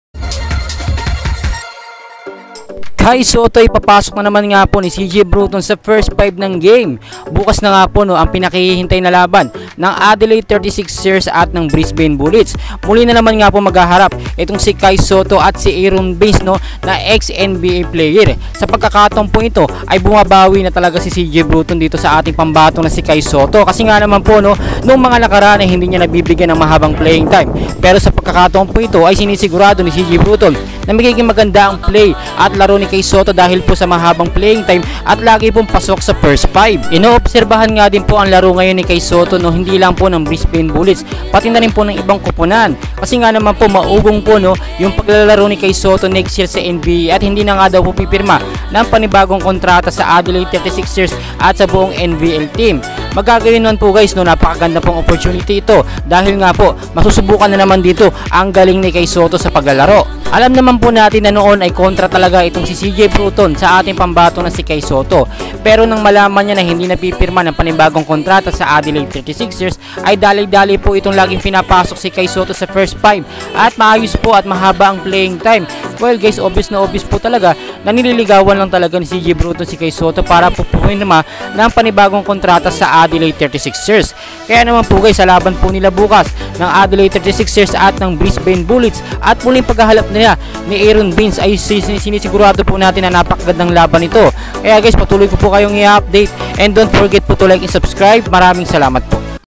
CJ BRUTON! Bumait Kay KAI SOTTO! 1ST FIVE NA! Long Playing Time pa!! ADL VS BULLETS!
kaisottoupdate